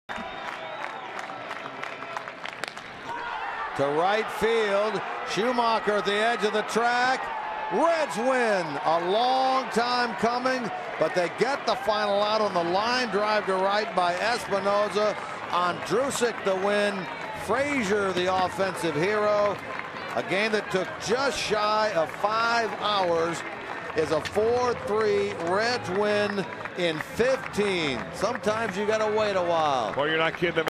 Ondrusek induces flyout for the save
5/19/14: Logan Ondrusek forces Danny Espinosa to fly out to right field, securing the Nationals' 4-3 win in the 15th inning